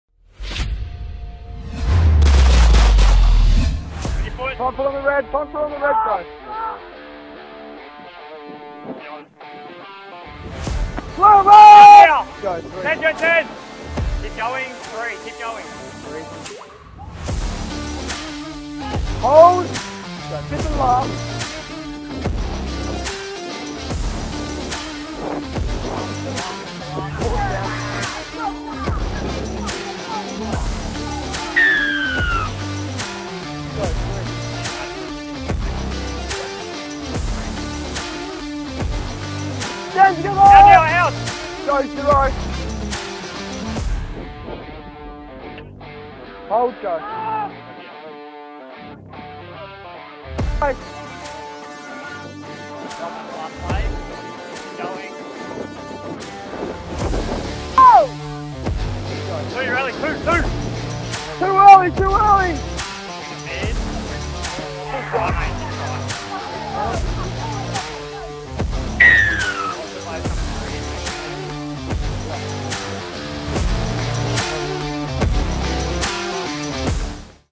Cyril Connell Cup Weapon: Round 2 - Santino Pekepo-Tevaga
Tweed Seagulls RLFC centre Santino Pekepo-Tevaga was hard to contain in his Round 2 Cyril Connell Cup clash, scoring a double to secure a win.